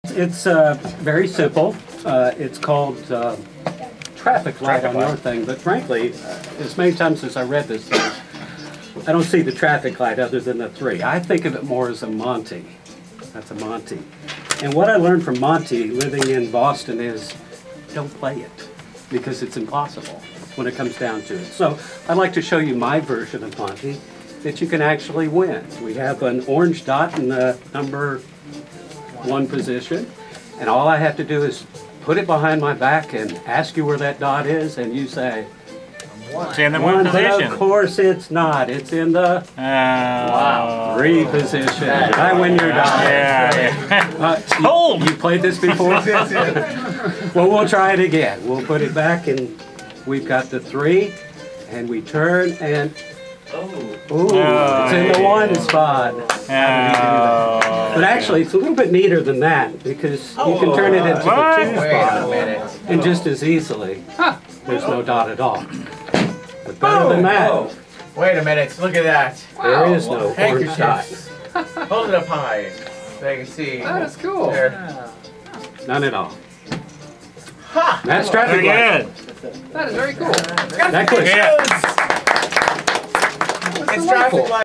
SS$ Nov 2014 Traffic Light
Second Saturday $ale November 2014 demonstration of the Traffic Light at Magic ETC / Fort Worth Costume.